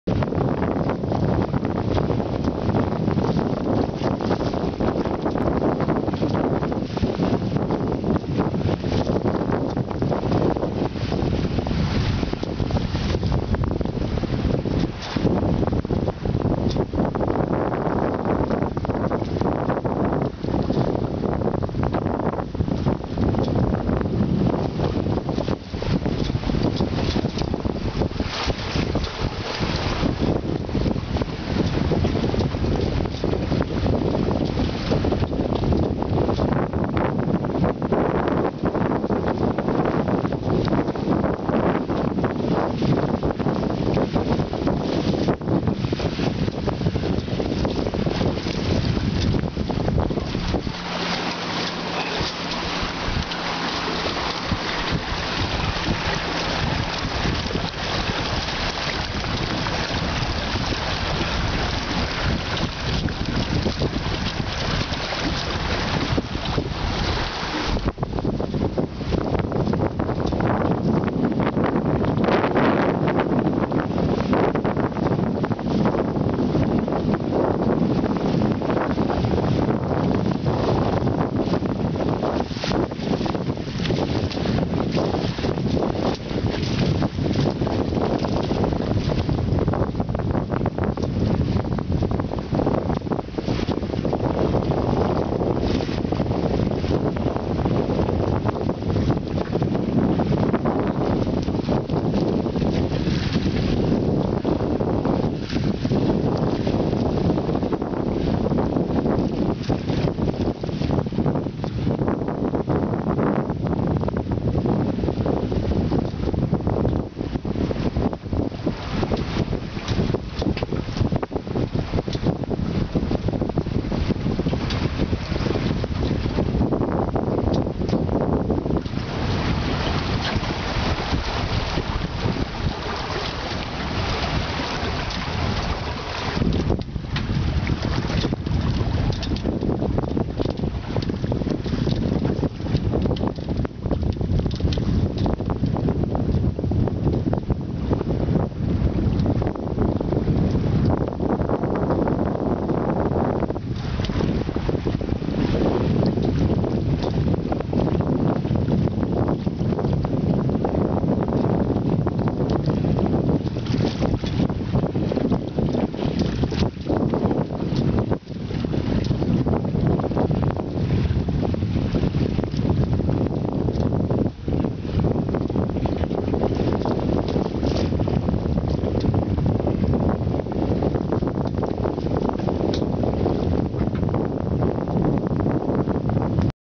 West Wight Potter 15
Potter sailing on Lake Margrethe in Grayling, Michigan